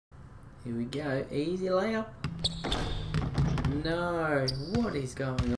Bobby Hurley How did u Miss
Some more season Highlights - Bobby Hurley loving the passing game!
Go you rockets